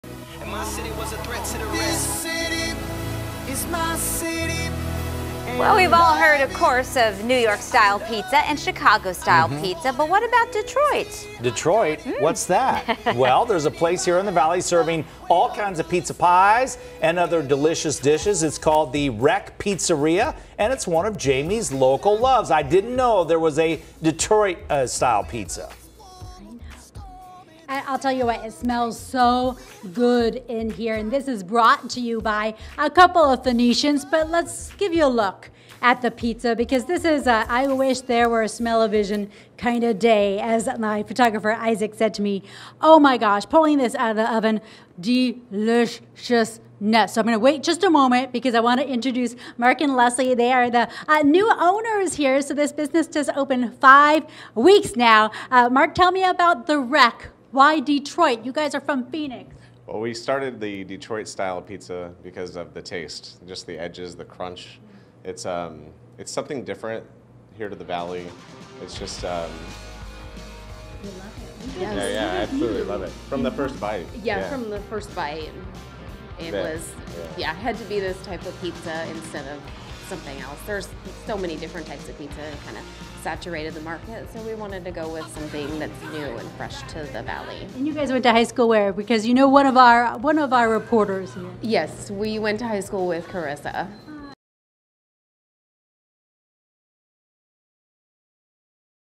Detroit-style pizza comes to the northwest Valley
In Jaime's Local Love this week, we check out Rec Pizza for some Detroit-style pie!